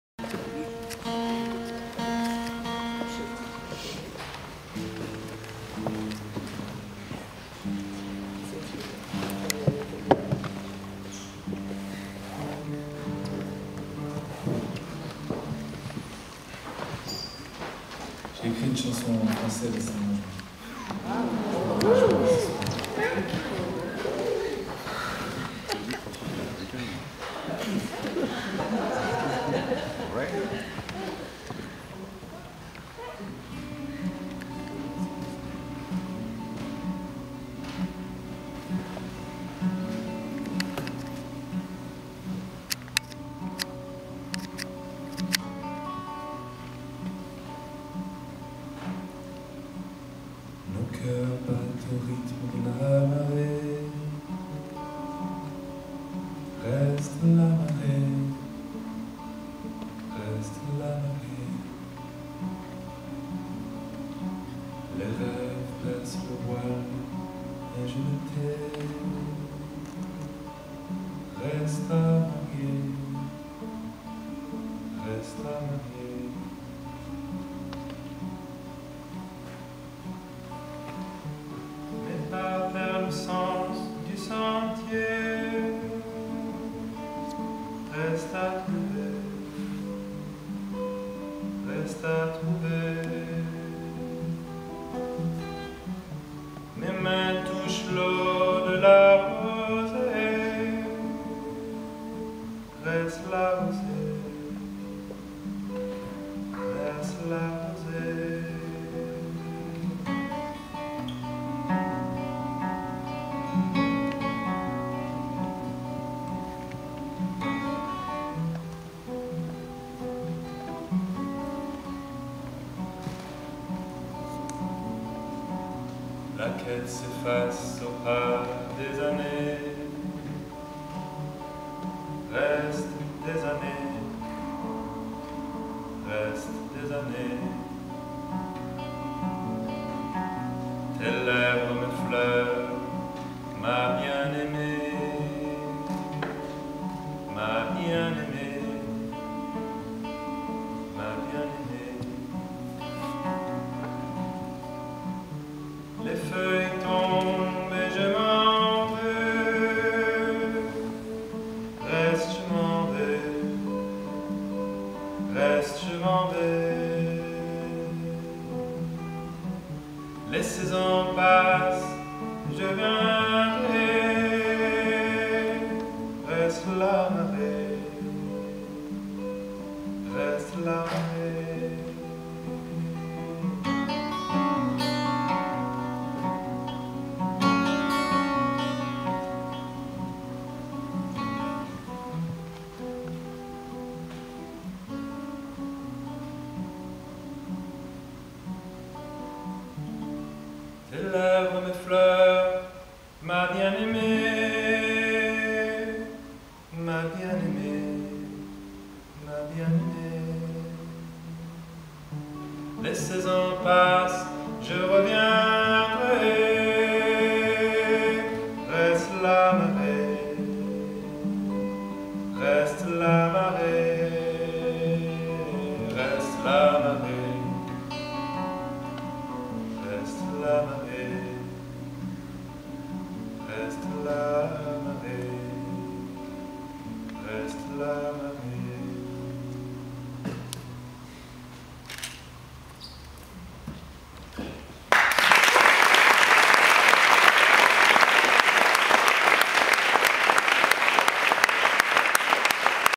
Piers Faccini aux Rendez-vous de la Lune 2012
Piers interprète un nouvelle chanson, écrite en français